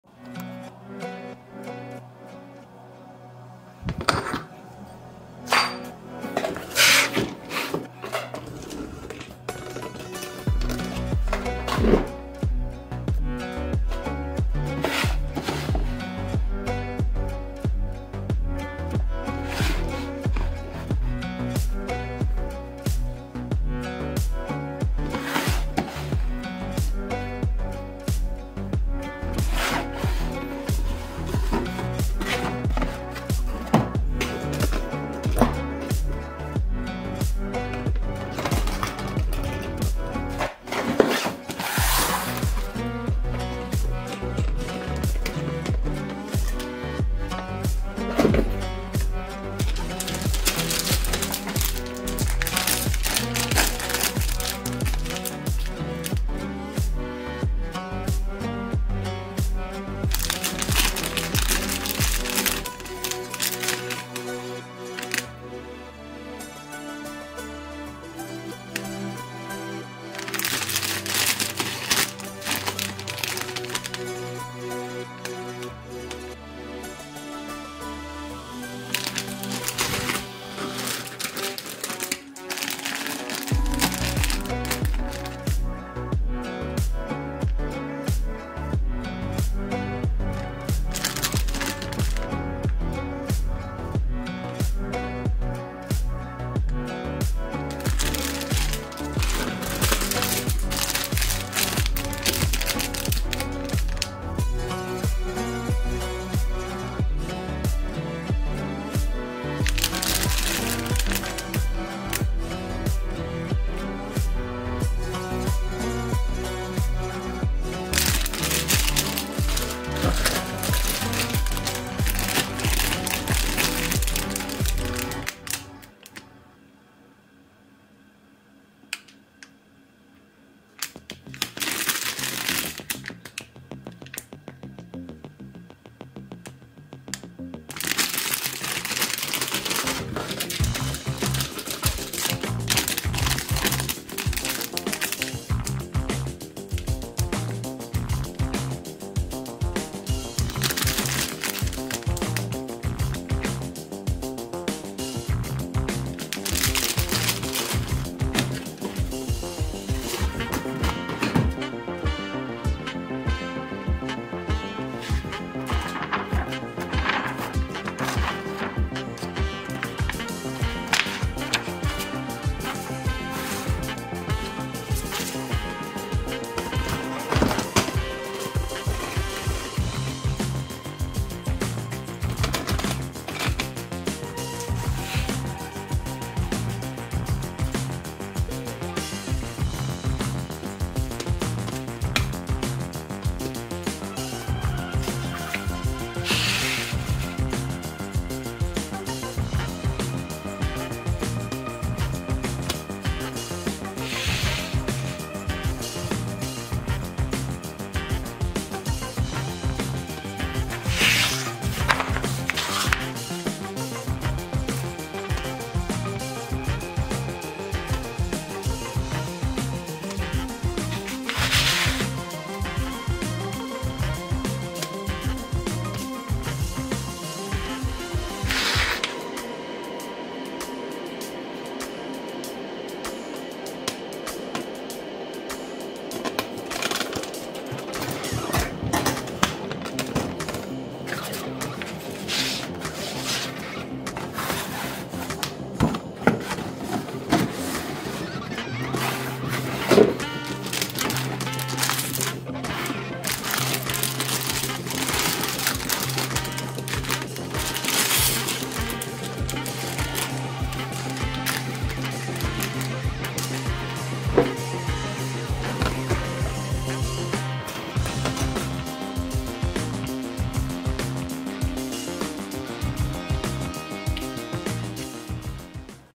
萬代HG 1/144 吉昂激流(艾格妮絲.吉本拉特專用機) -機動戰士鋼彈SEED FREEDOM系列快速開箱!GYAN Strom ギャンシュトローム
超期待終於來一了新妹子隊友!!結果竟然是雷包!!
4K高畫質開箱!